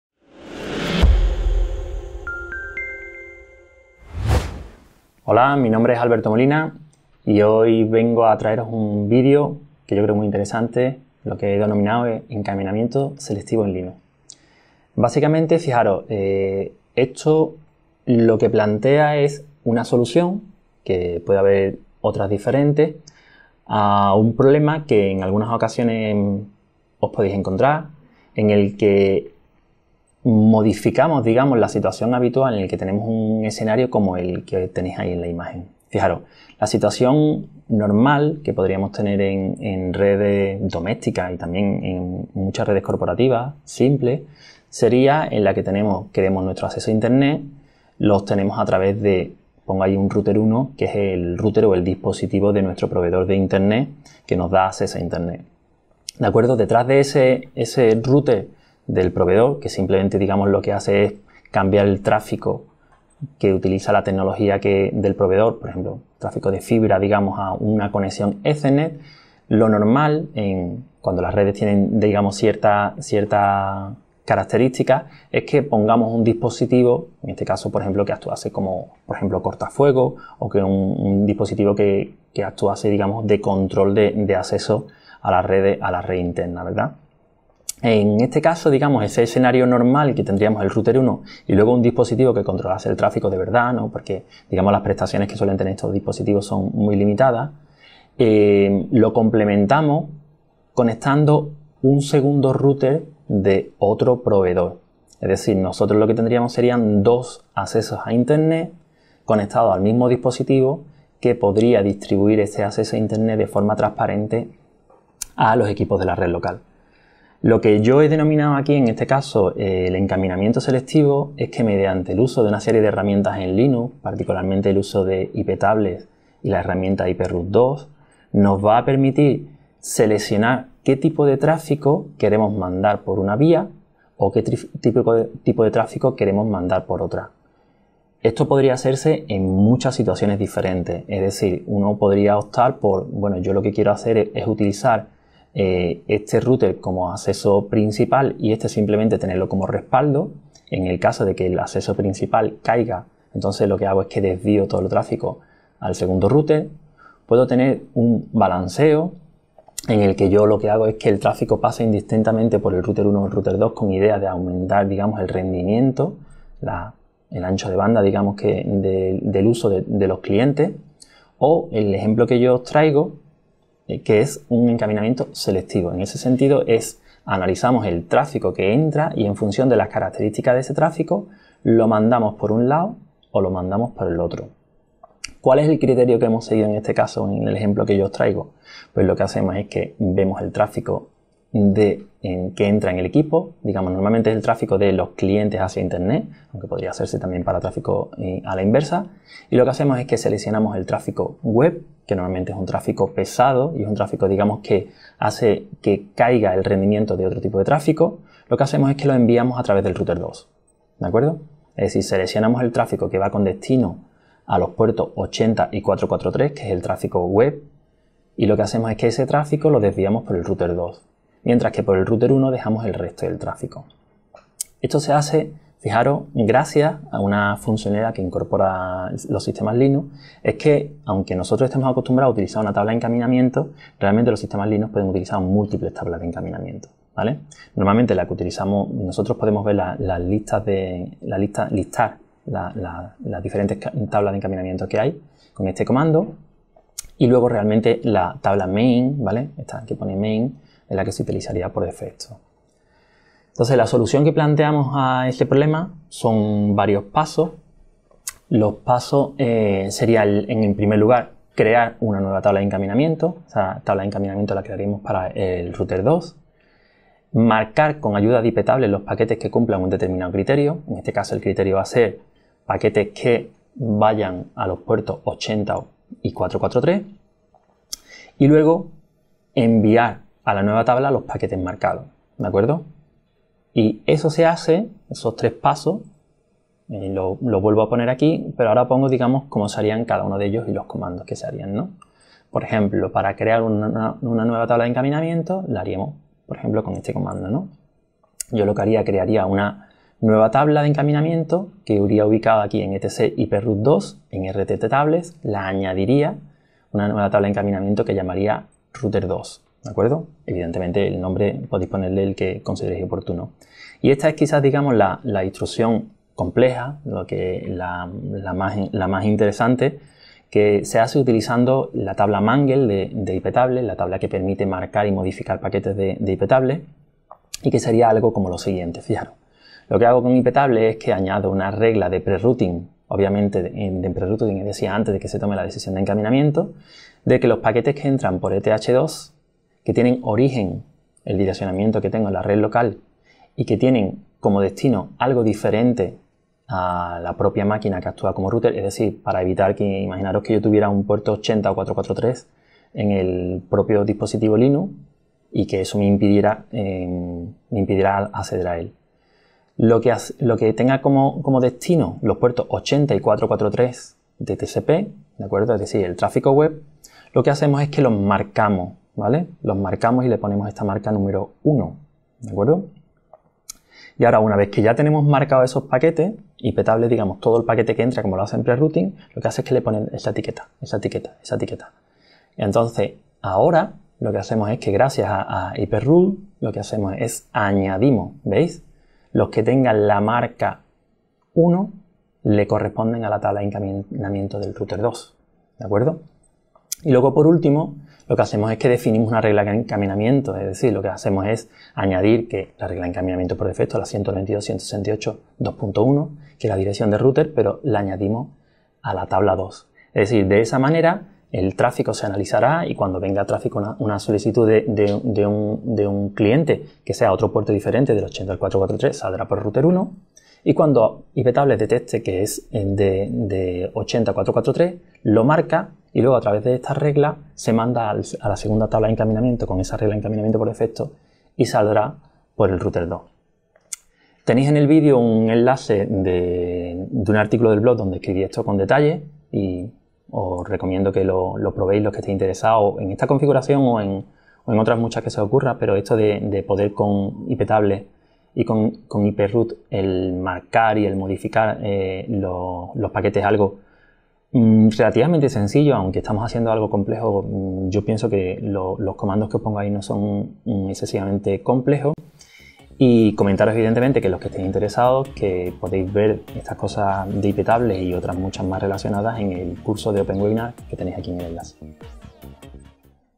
ENRUTAMIENTO SELECTIVO EN LINUX
ÍNDICE:
1 # 0:14 ¿Qué es el enrutamiento selectivo en Linux?
2 # 3:44 ¿Cómo se hace el enrutamiento selectivo?
3 # 4:12 ¿Cuál es la solución?